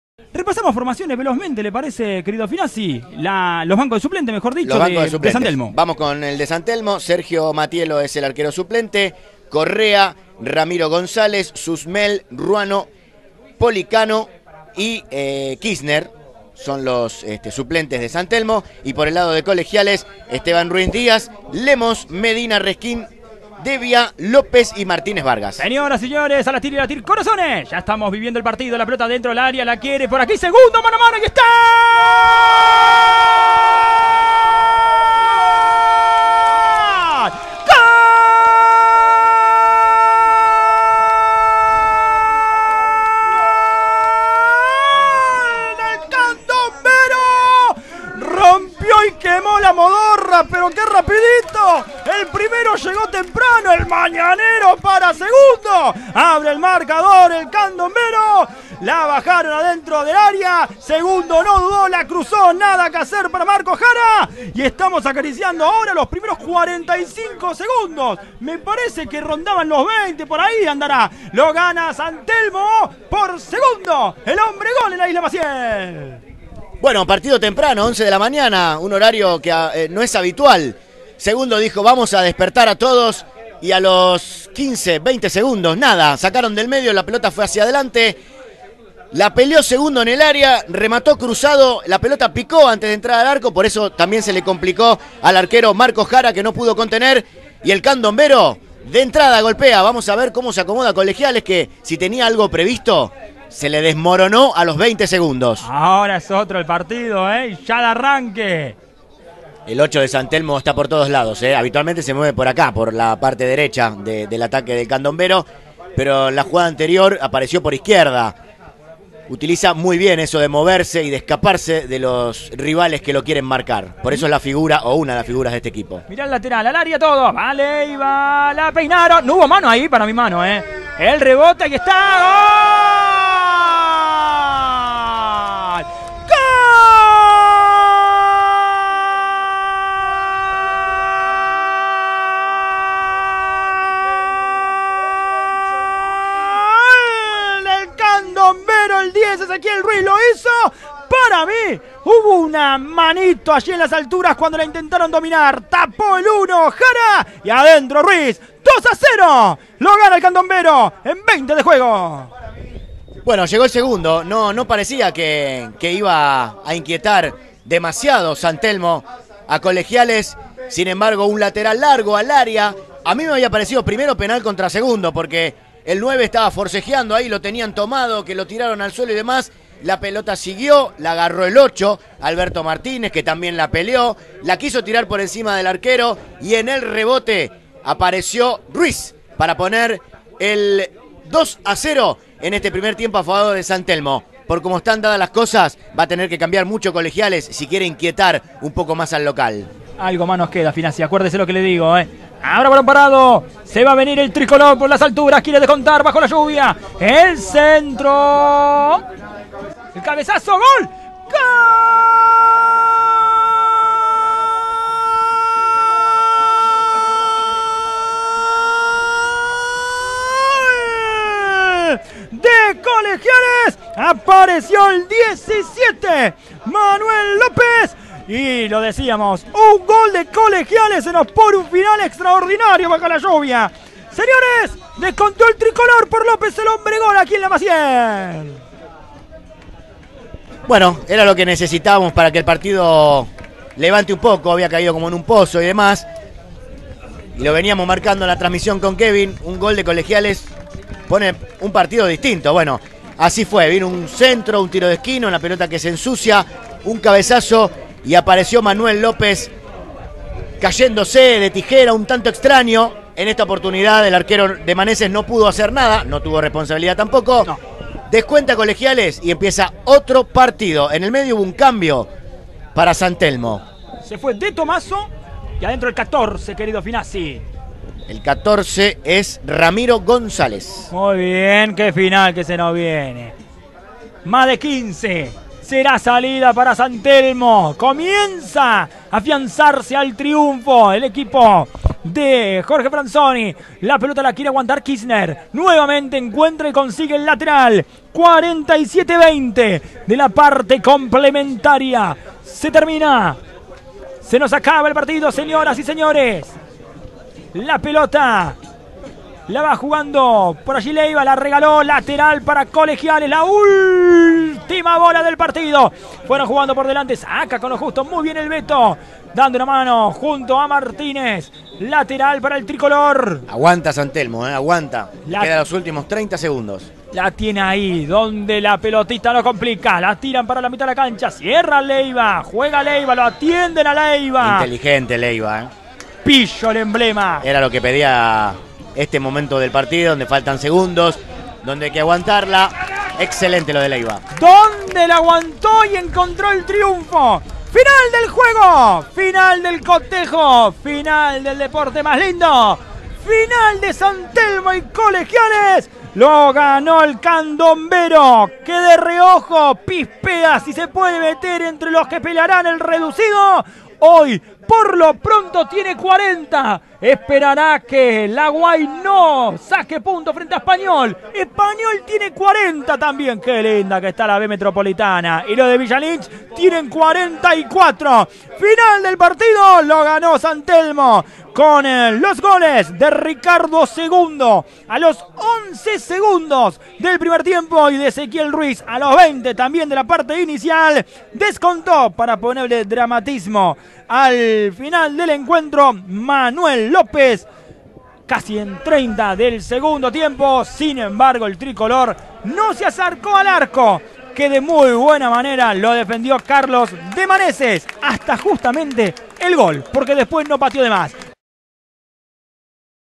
San Telmo 2 Colegiales 1 (Relato Kevin Ternavasio) Primera B Metro 2017
Los goles del partido con relato de Kevin Ternavasio Tyc Sports Play